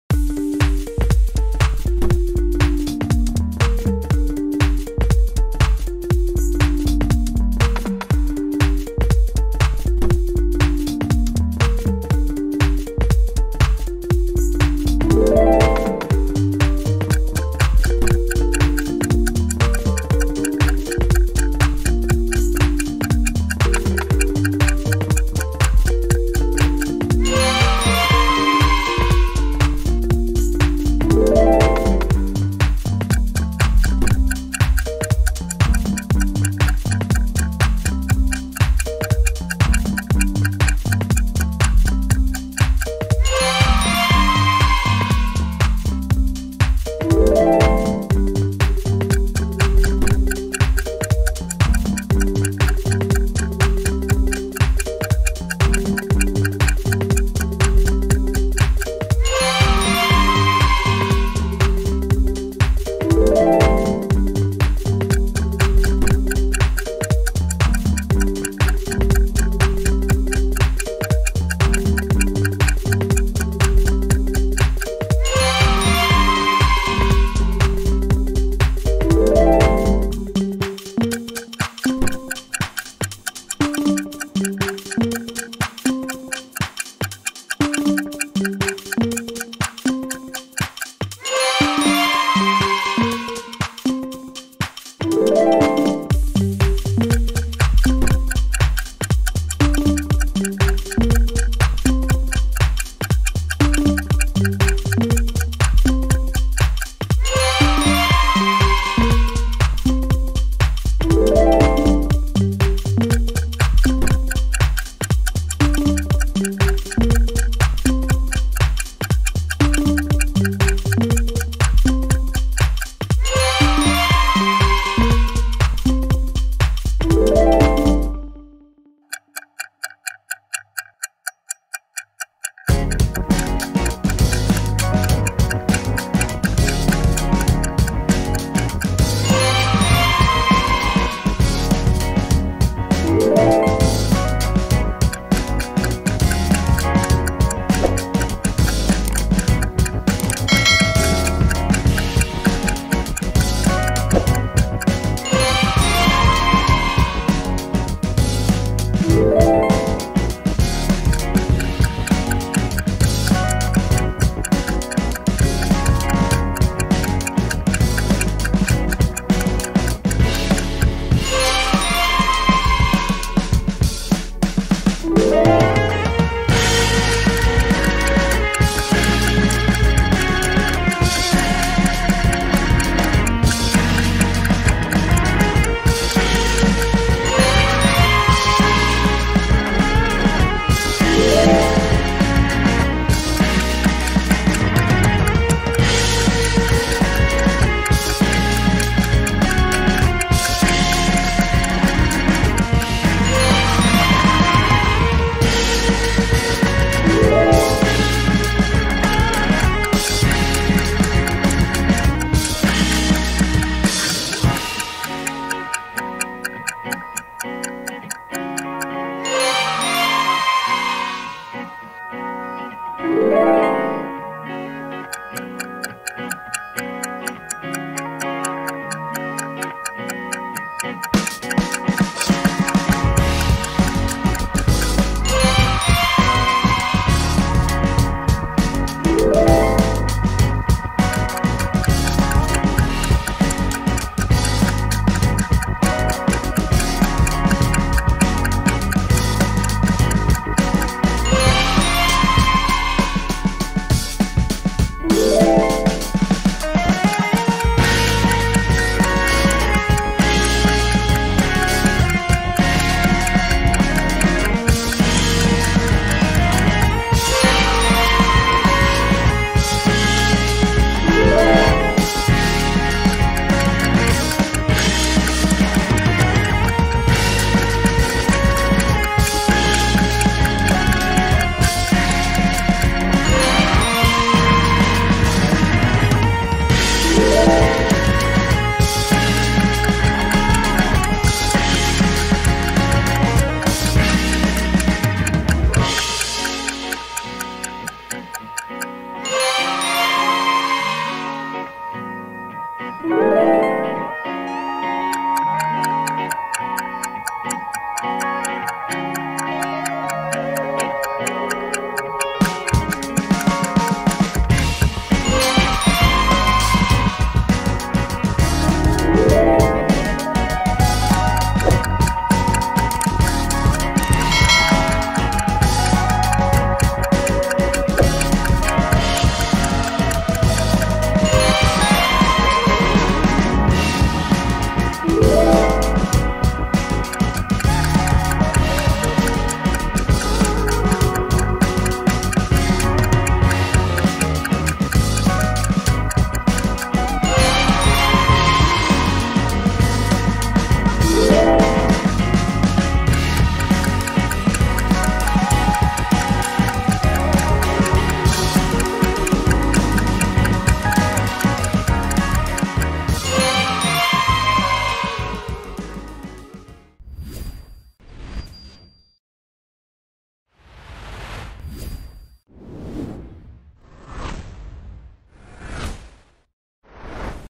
Grammar Quiz : Subordinate Conjunctions (Before, After, As soon as, until, while)
Hey guys!
Today I’m going to show you 22 Simple Grammar Questions. Each questions have 15 sec. Answer all the questions and comment your mark below.